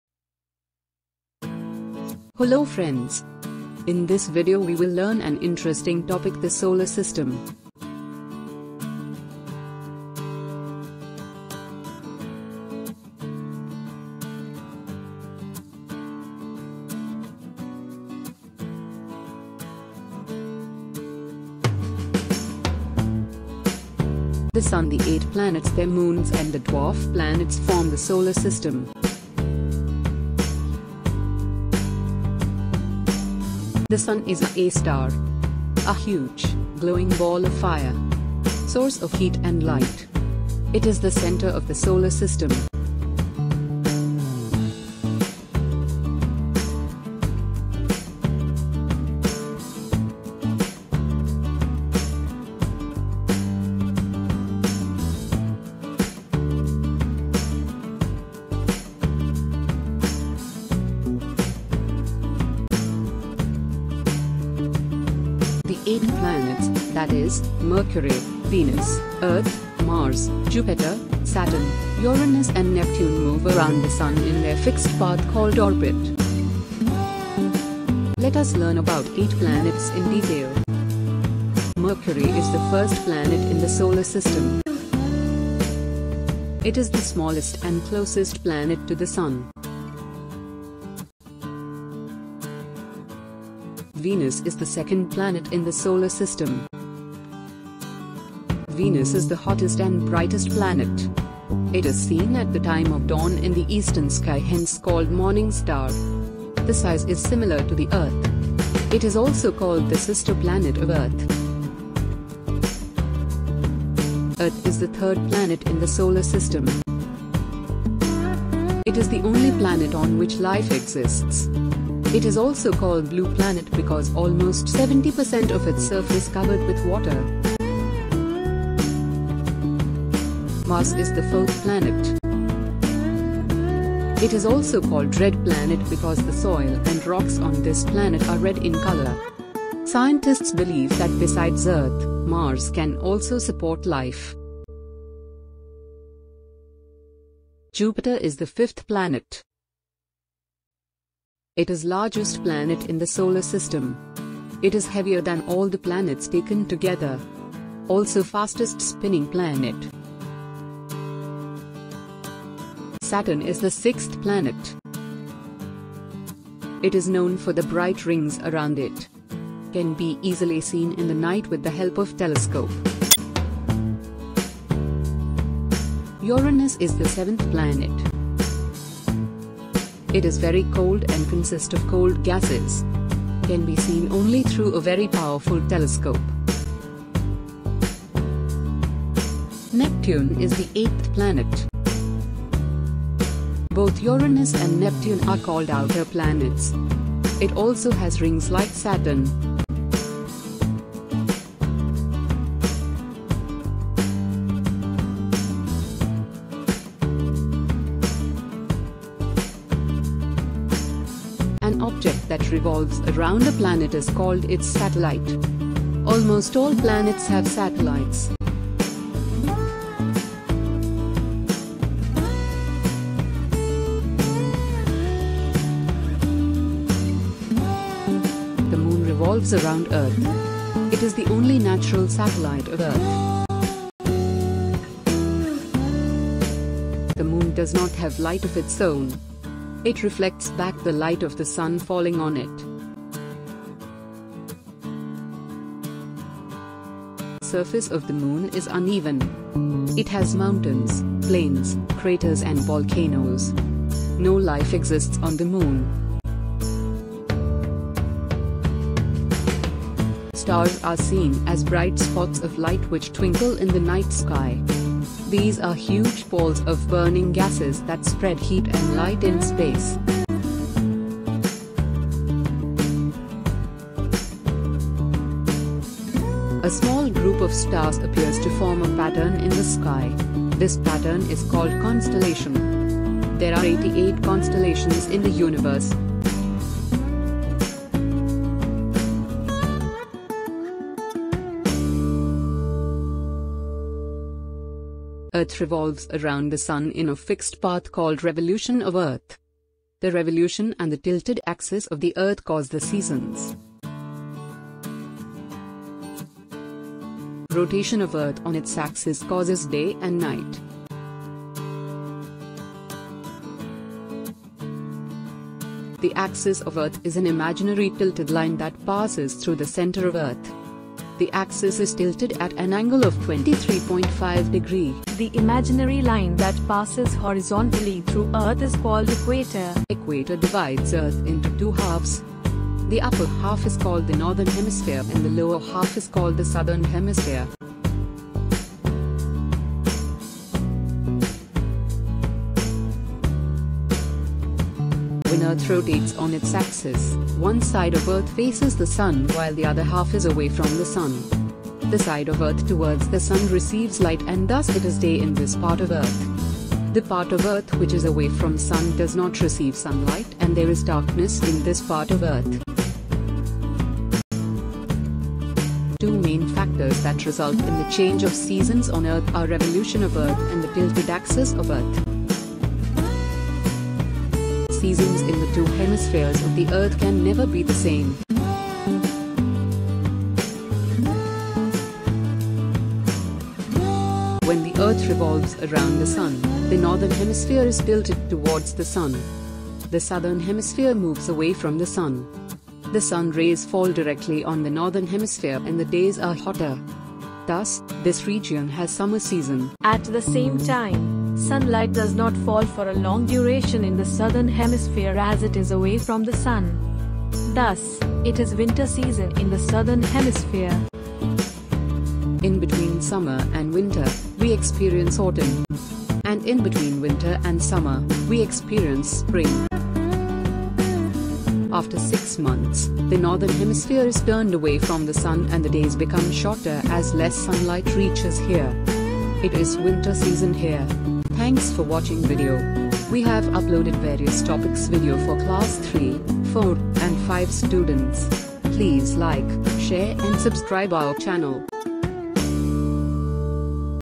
The Solar System | Whats in are solar system? Class 4| Science
What's in are solar system?
CBSE | NCERT | Science | The Solar System | Class 4 |

Video content :
Solar system
Planets
Moon and satellites
Stars
Rotation and Revolution of Earth
Day and Night
Seasons